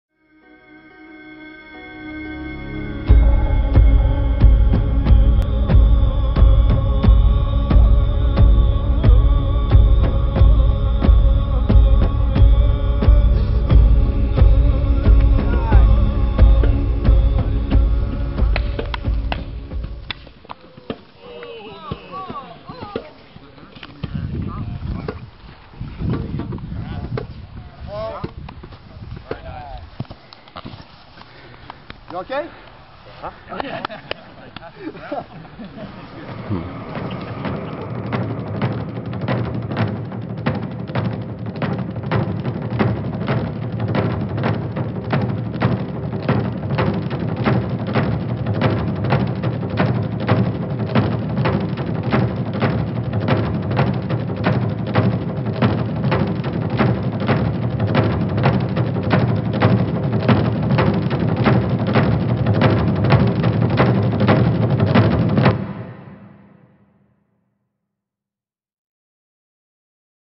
Eric vs Charles-Andre - Single Stick - Dog Brothers Training Camp - August 23 - 2009
Gelinas Academy of Mixted Martial Arts
1117 Ste-Catherine West,
2nd floor #222
Montreal, QC, Canada, H3B 5K2